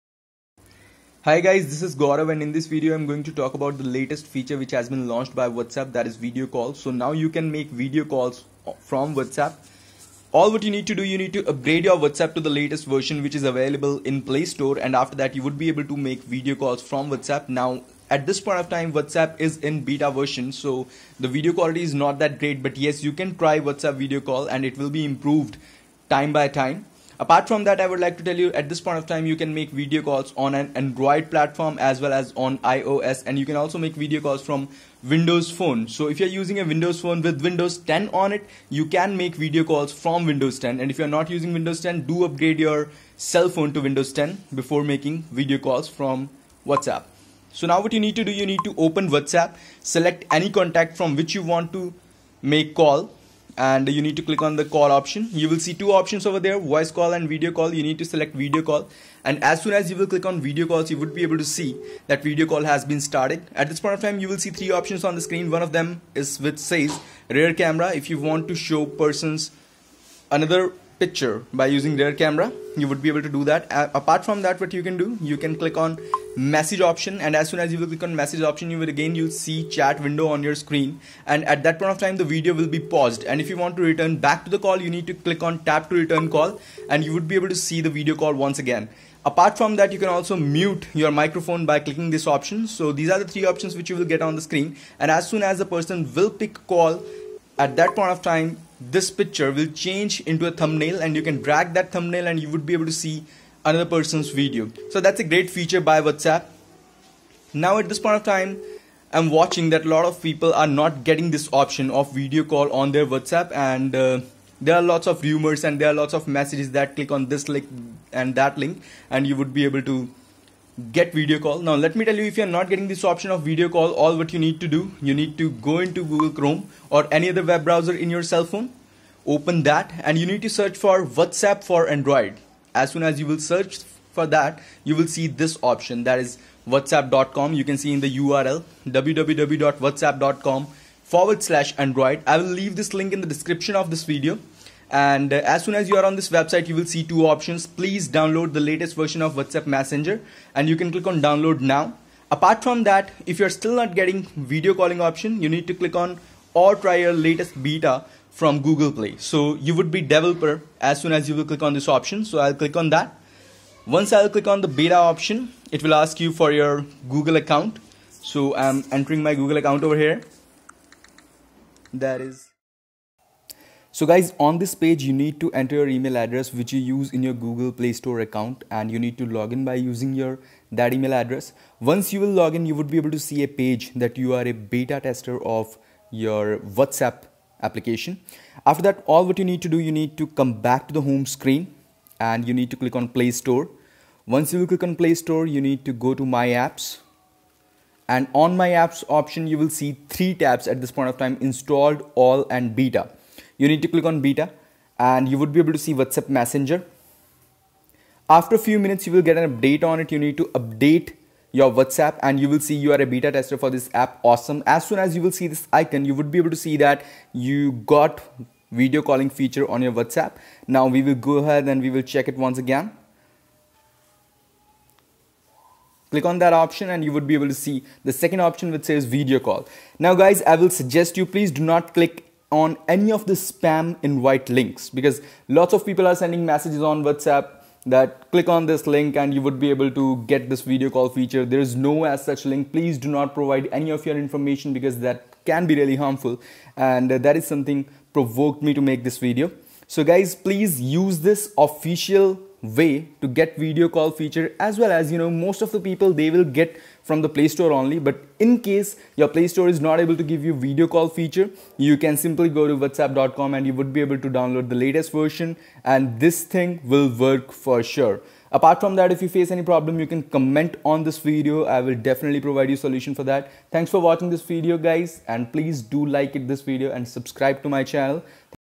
WhatsApp Video Call Demo and Steps to Get Video Call Feature on WhatsApp
In this Video, You would be able to see the demo of Whatsapp Video Call. Along with that You will also see steps to get Video Call feature on WhatsApp.

Steps:-
1. Open Any web browser
2. Search for Whatsapp for Android
3. Open link and download the latest version OR Click on "BETA" to become the developer and to get all the frequent updates for WhatsApp.
5. Sign in and you will see a message. "Congratulations, You are a tester of Whatsapp.
6. Go to Playstore.
7. Open MY APPS, Click on BETA (Wait for few minutes)
8. Update Whatsapp and you will get video calling feature.